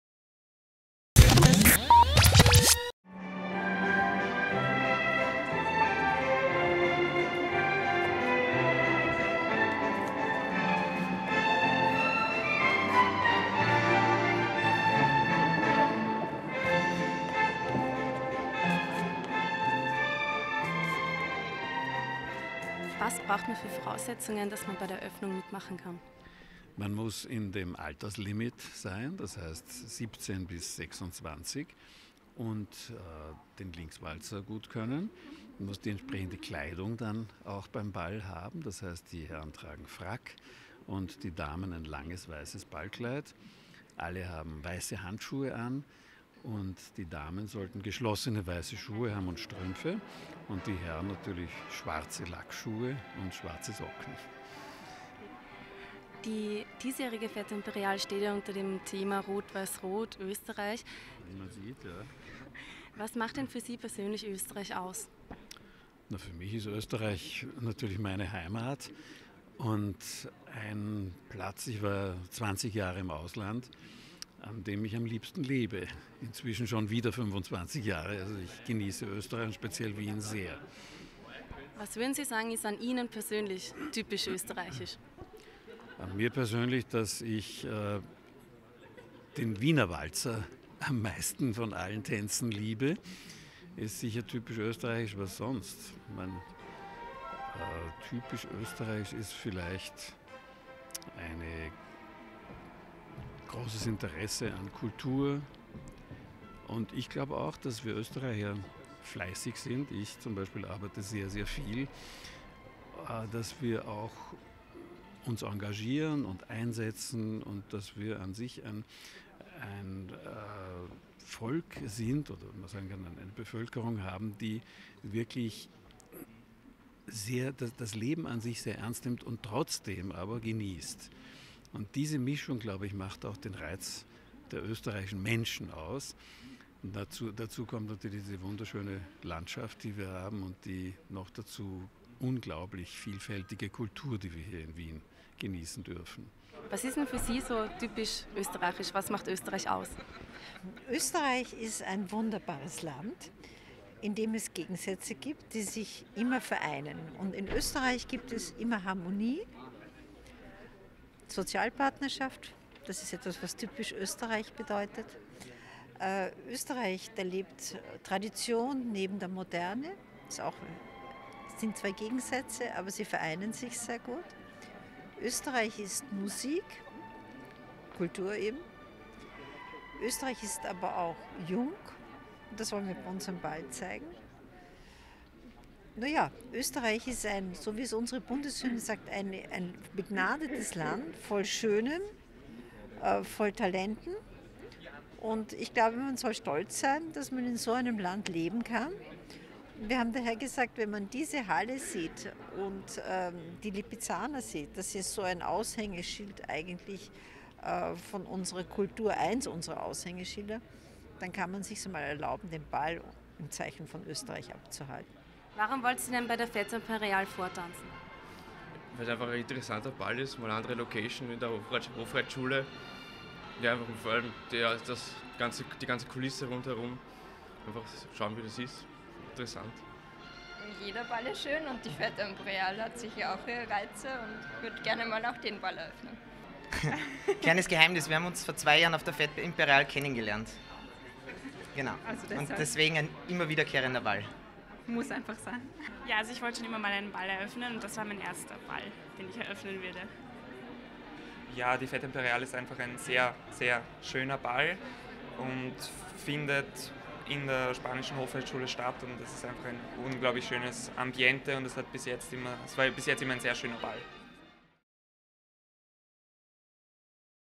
Vortanzen für die Fête Impériale
Am Montag, 3. Juni, wurde in der Spanischen Hofreitschule um 18.00 Uhr vorgetanzt für die Fête Impériale. Wir befragten Tanzlehrer Thomas Schäfer-Elmayer und Ball-Organisatorin Elisabeth Gürtler, passend zum Ball Motto, nach ihren typischen österreichischen Eigenschaften.